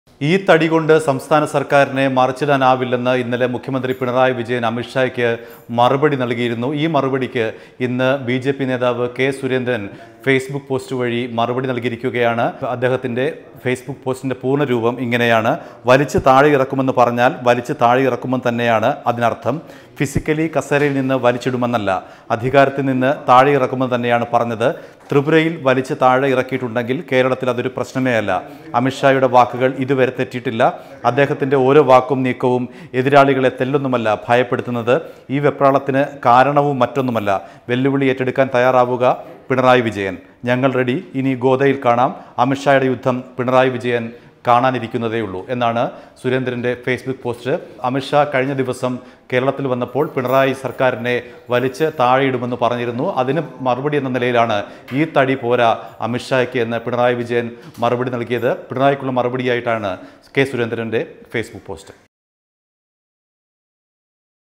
ത്രിപുരയില്‍ വലിച്ചു താഴെയിട്ടെങ്കില്‍ കേരളത്തിലും വലിച്ചു താഴെയിടും | K Surendran
സര്‍ക്കാരിനെ വലിച്ച് താഴെയിറക്കുമെന്ന് പറഞ്ഞാല്‍ അതിന് മറ്റൊരു അര്‍ത്ഥമില്ലെന്ന് കെ സുരേന്ദ്രന്‍. ത്രിപുരയില്‍ ആവാമെങ്കില്‍ ബിജെപിക്ക് കേരളത്തിലുമാകാമെന്ന് സുരേന്ദ്രന്റെ ഫെയ്‌സ് ബുക്ക് പോസ്റ്റ്